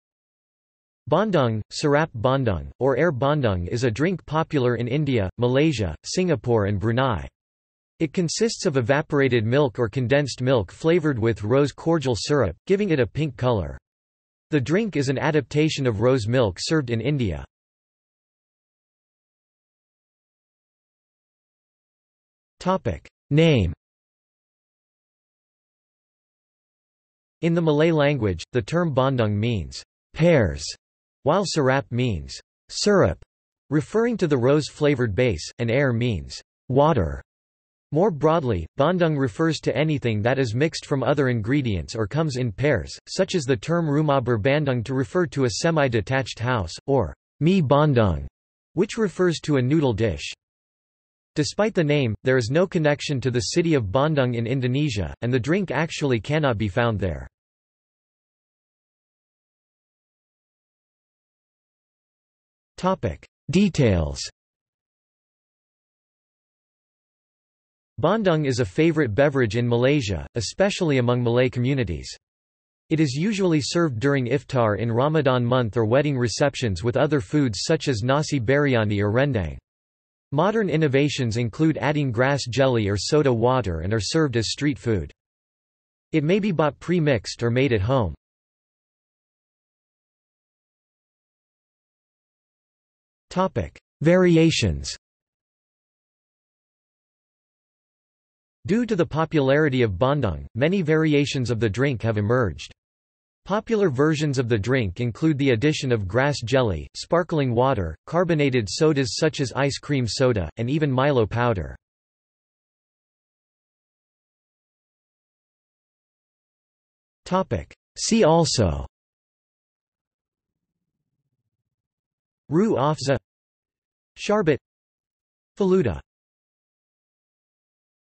Bandung (drink) | Wikipedia audio article
This is an audio version of the Wikipedia Article:
Bandung (drink)

00:00:19 1 Name
00:00:59 2 Details
00:01:29 3 Variations
00:01:53 4 See also

- Socrates

SUMMARY
Bandung, sirap bandung, or air bandung is a drink popular in India, Malaysia, Singapore and Brunei. It consists of evaporated milk or condensed milk flavoured with rose cordial syrup, giving it a pink colour. The drink is an adaptation of rose milk served in India.